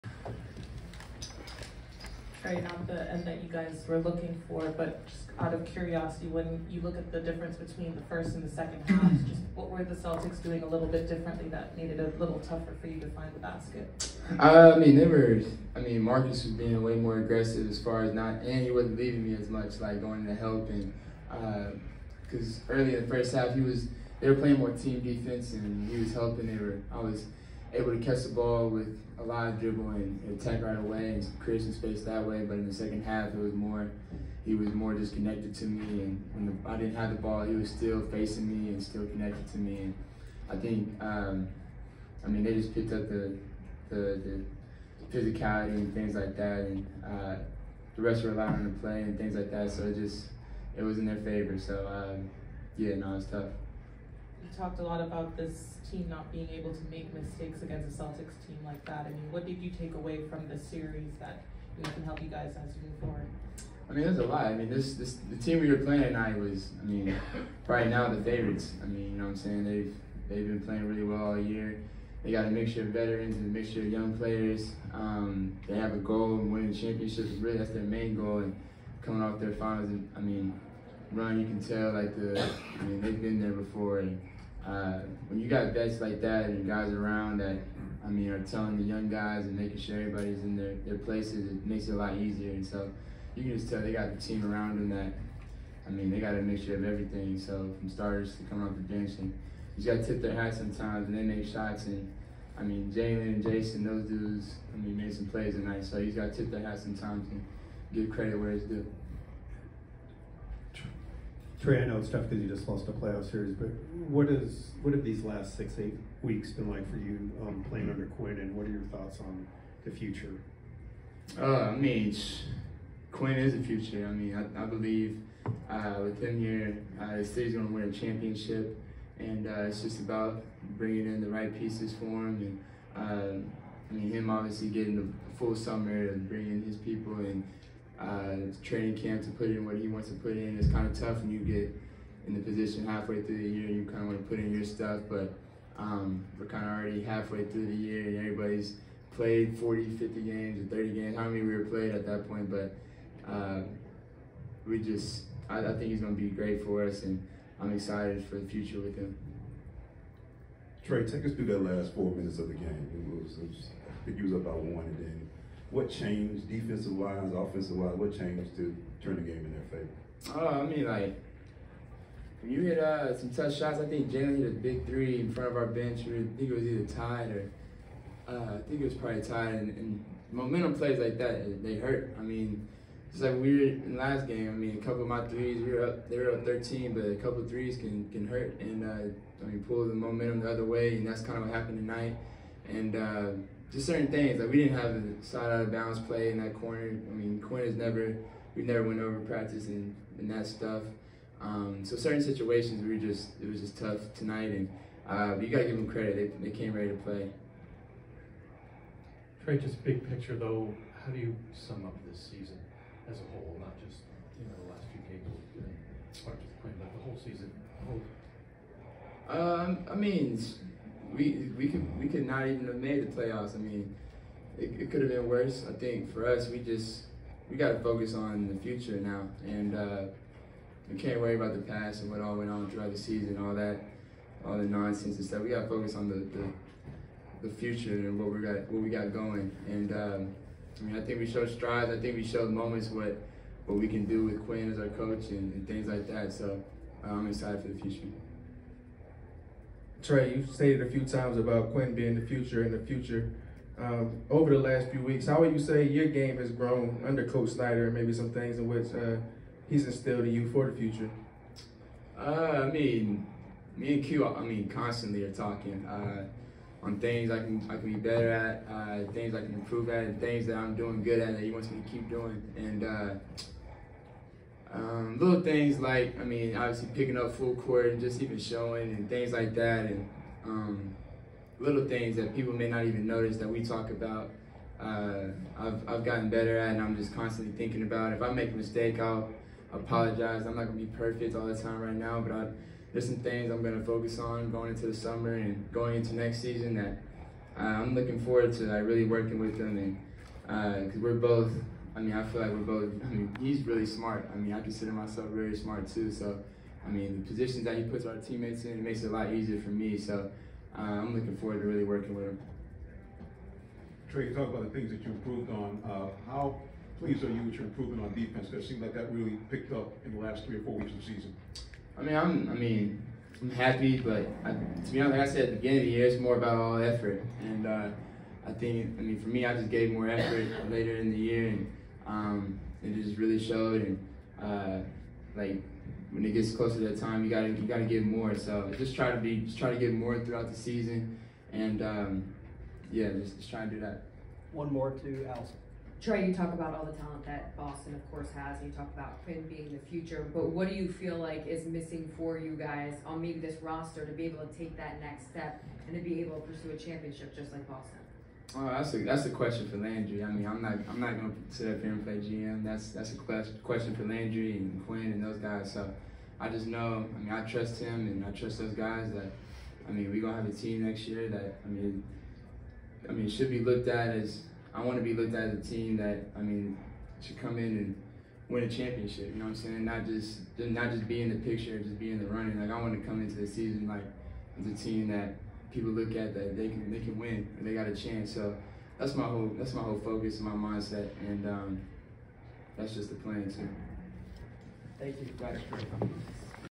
04-27-23 Atlanta Hawks Guard Trae Young Postgame Interview
Atlanta Hawks Guard Trae Young Postgame Interview after losing to the Boston Celtics at State Farm Arena.